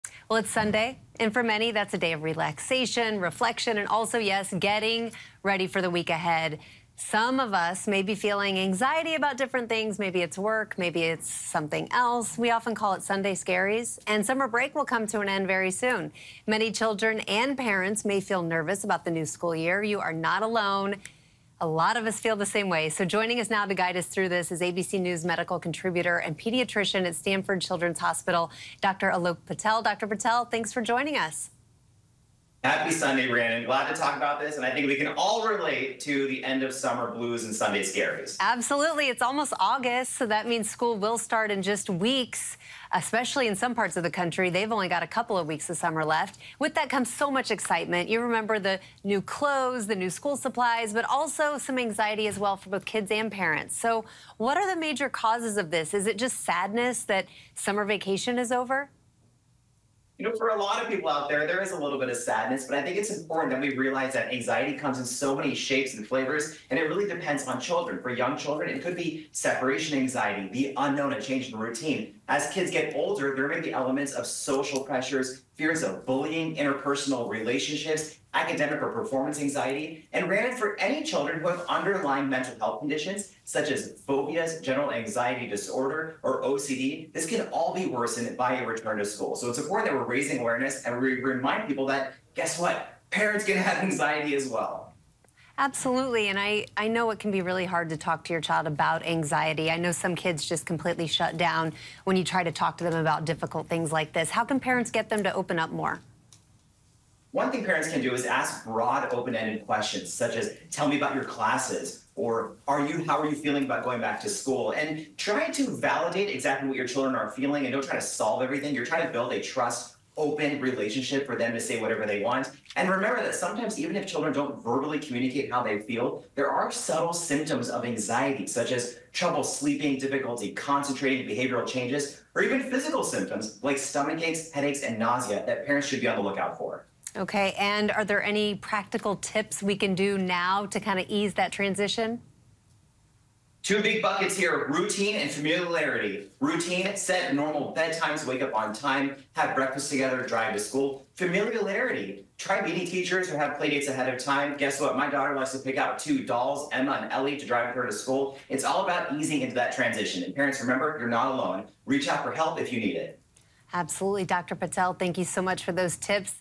How to handle back-to-school anxiety
ABC News’ Dr. Alok Patel gives tips on how parents and children can handle the “summer blues” and recognize the signs of anxiety that may present themselves leading up to a new school year.